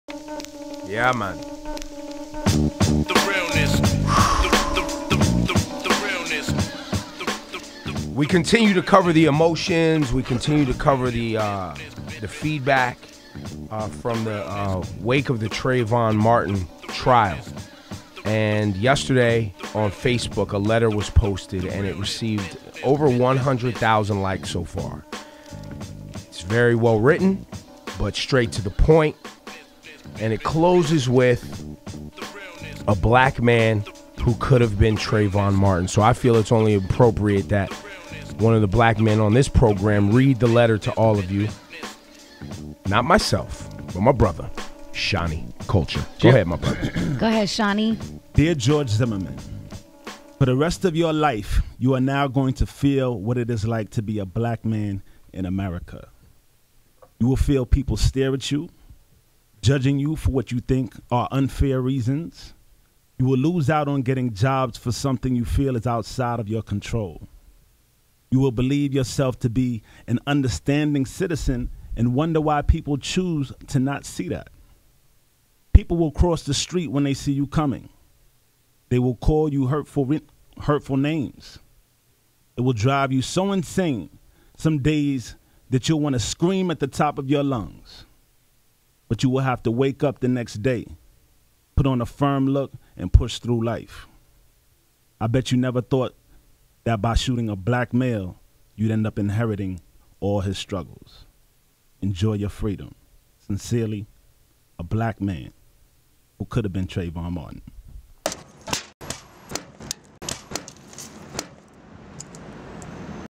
THE REALNESS: A letter to George Zimmerman
Shani Kulture Reads A Letter To George Zimmerman